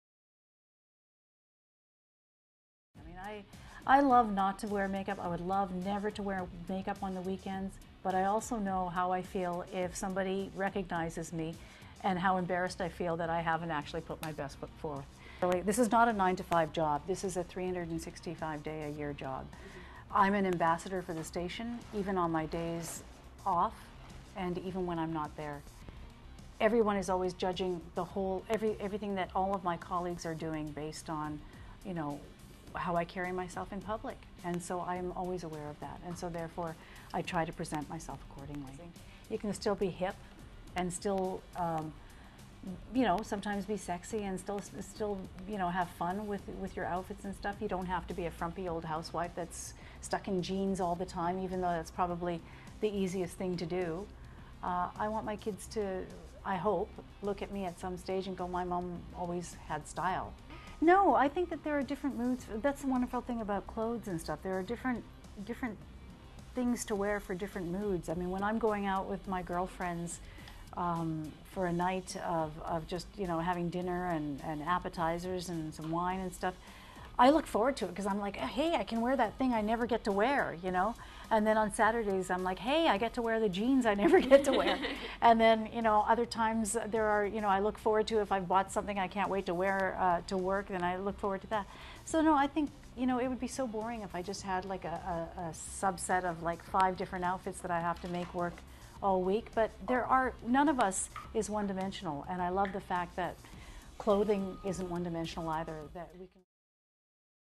CBC Calgary news anchor Nirmala Naidoo
News Flash: In this Dec. 2009 Style Q+A with Avenue Magazine Calgary, Naidoo redefines timeless elegance.